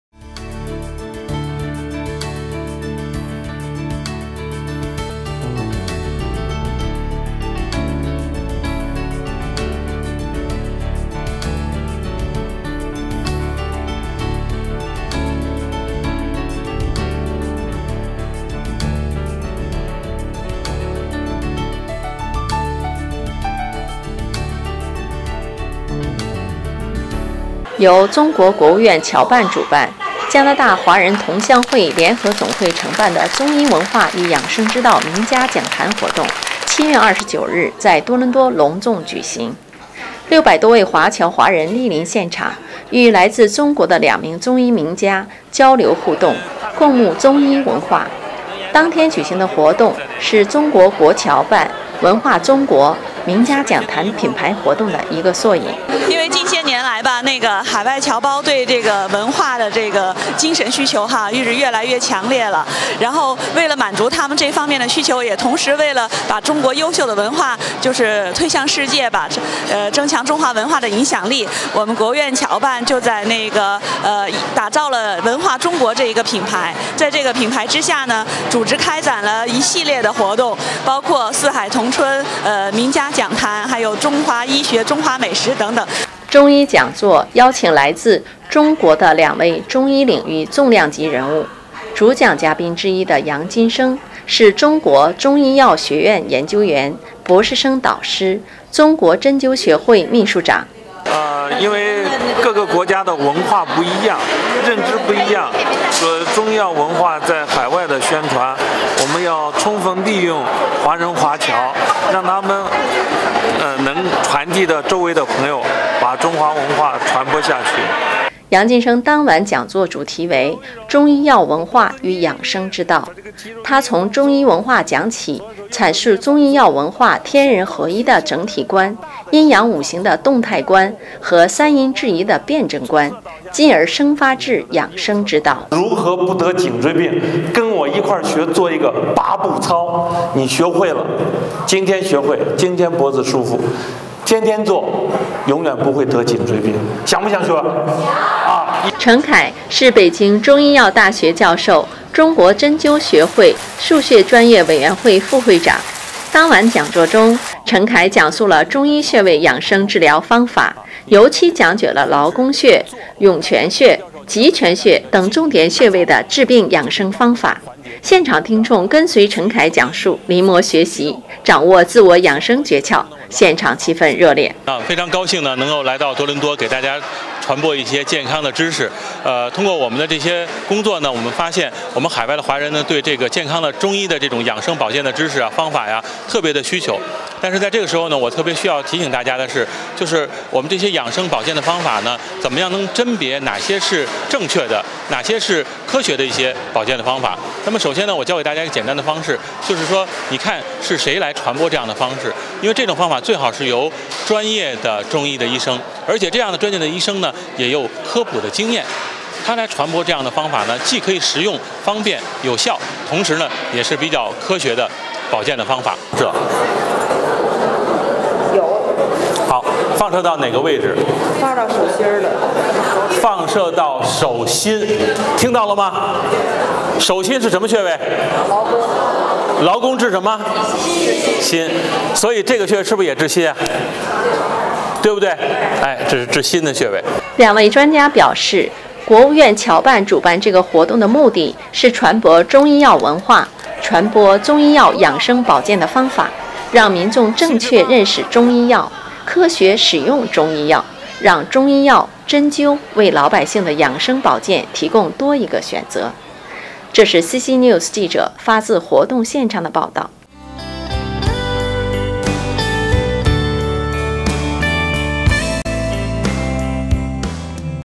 中医文化名家讲坛活动受海外华人追捧
由中国国务院侨办主办、加拿大华人同乡会联合总会承办的中医文化与养生之道名家讲坛活动，7月29日在多伦多隆重举行。六百多位华侨华人莅临现场，与来自中国的两名中医名家交流互动，共沐中医文化。当天举行的活动是中国国侨办《文化中国 名家讲坛》品牌活动的一个缩影。

中医讲座邀请来自中国的两位中医领域重量级人物。主讲嘉宾之一的杨金生是中国中医科学院研究员、博士生导师、中国针灸学会秘书长。杨金生当晚讲座主题为《中医药文化与养生之道》，他从中医文化讲起，阐述中医药文化天人合一的整体观、阴阳五行的动态观和三因制宜的辩证观，进而生发至养生之道。

程凯是北京中医药大学教授、中国针灸学会腧穴专业委员会副会长。当晚讲座中，程凯讲述了中医穴位养生治疗方法，尤其讲解了劳宫穴、涌泉穴、极泉穴等重点穴位的治病养生方法，现场听众跟随程凯讲述，临摹学习，掌握自我养生诀窍。现场气氛热烈。